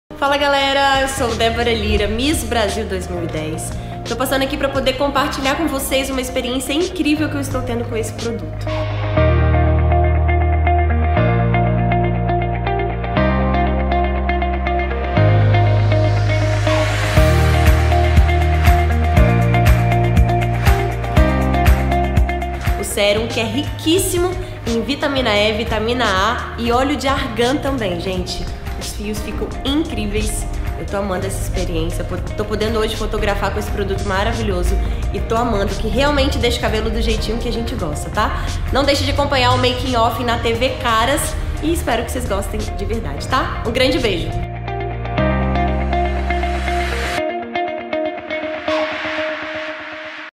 Debora Lyra, modelo e ex-Miss Brasil.
É uma honra poder representar esse produto que chegou com tudo no Brasil ✔️ Atenção mulherada: já pode colocar o @mhairserum serum na sua rotina capilar, prometo que gostarão demais. 😍